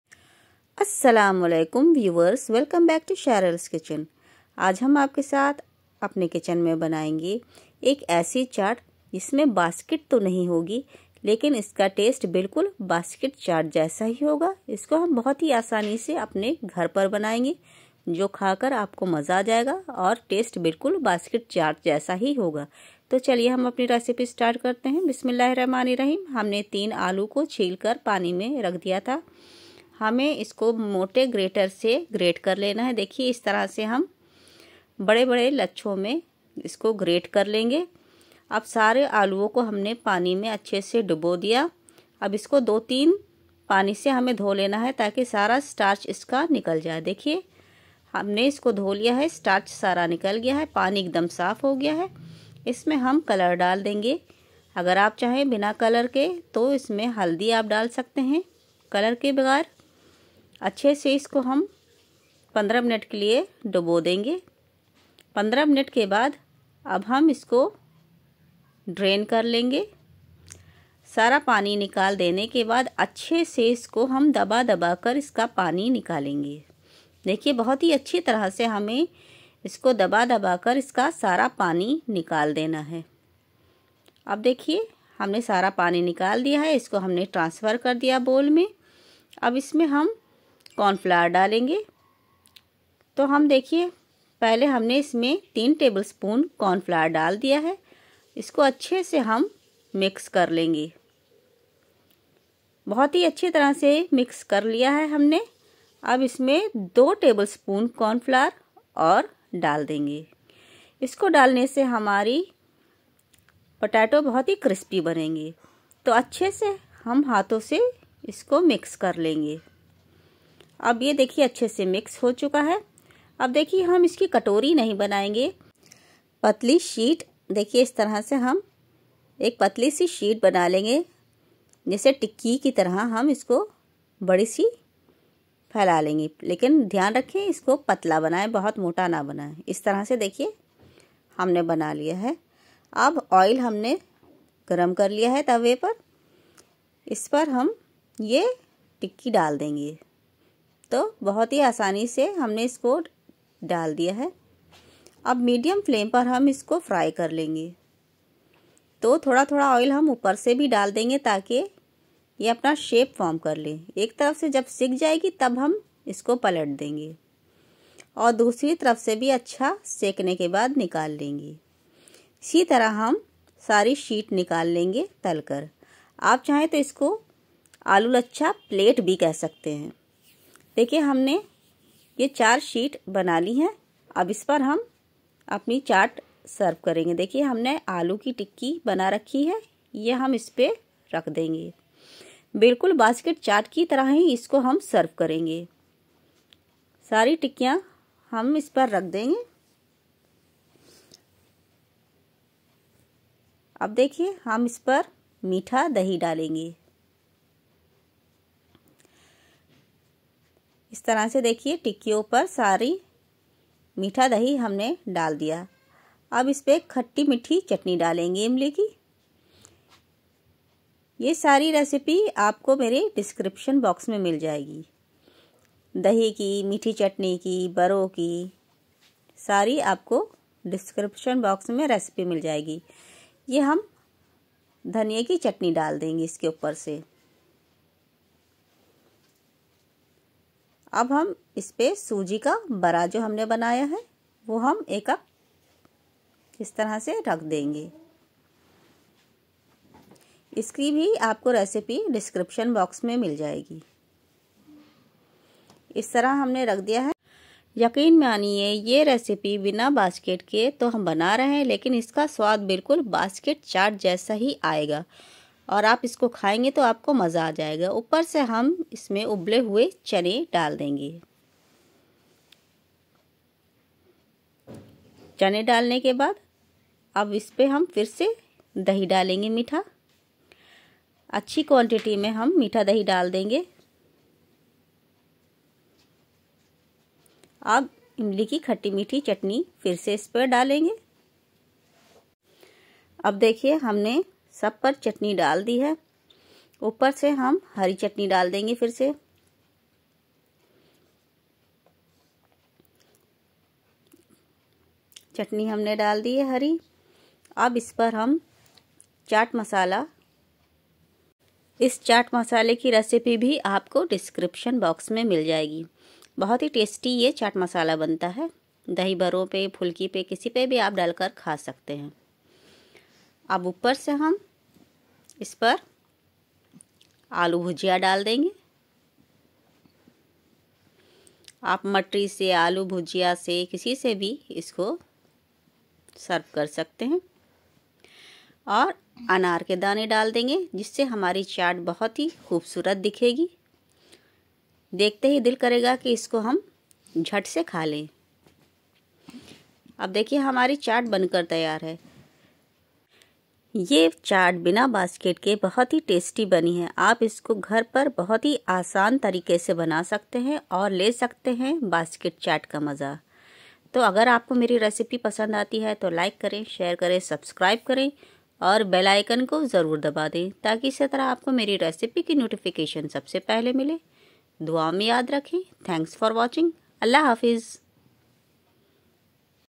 बिना बास्केट के बास्केट चाट का मजा लेना है तो इस रेसिपी को जरूर ट्राई करें |Basket chaat new version
basket for basket chaat.